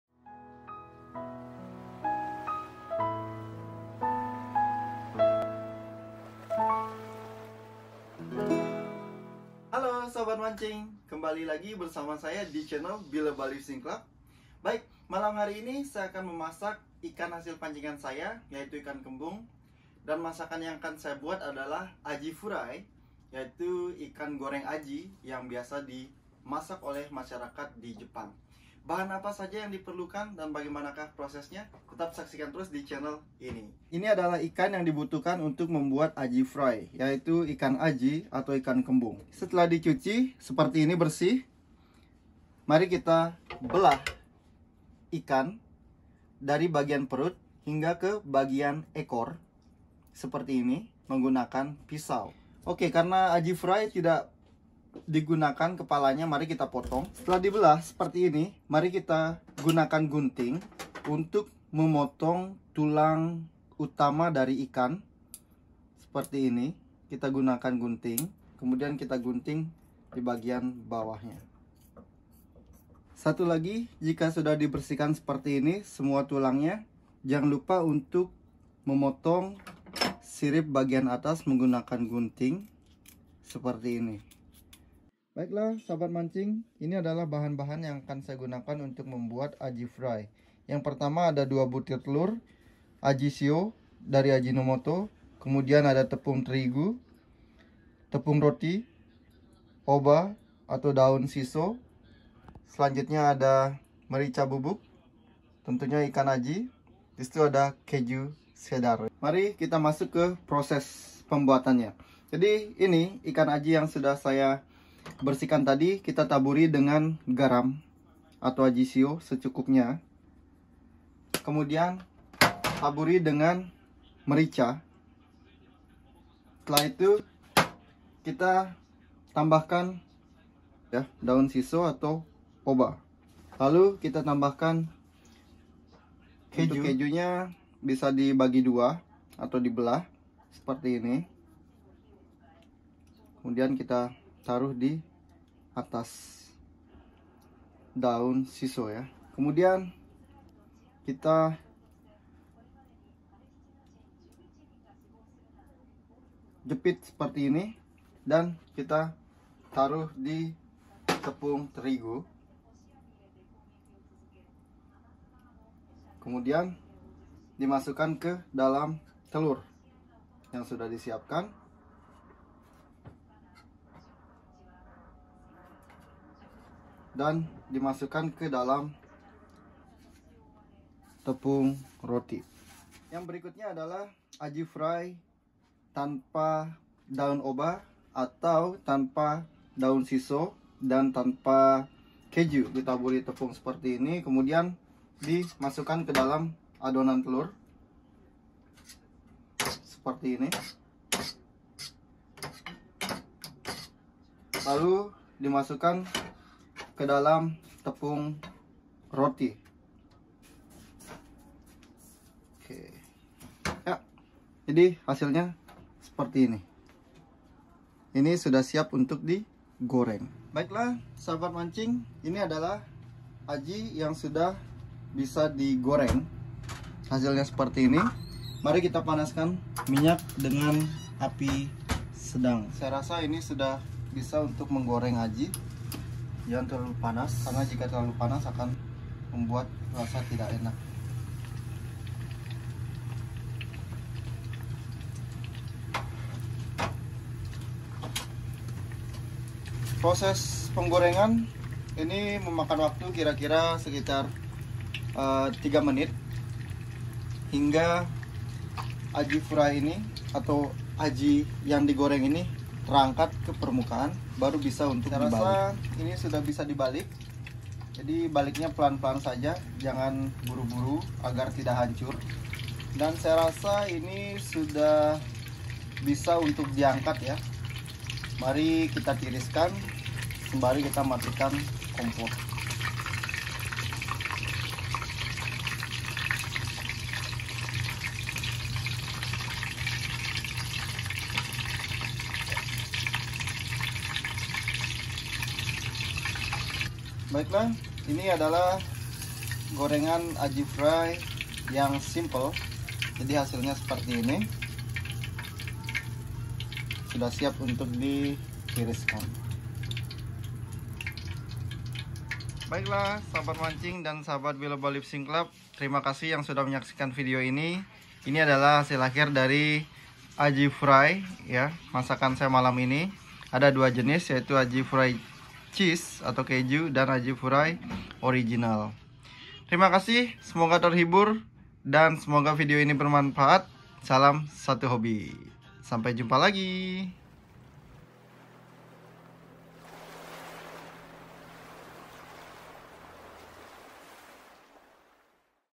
鯵フライの作り方 | MAKING JAPANESE AJI FRY | Catch and Cook
Aji fry is one of delicious and famous fish fry in Japan. Most Japanese like to eat Aji furai. Have you try it before? Here the recipe and how to cook Aji Furai that i know.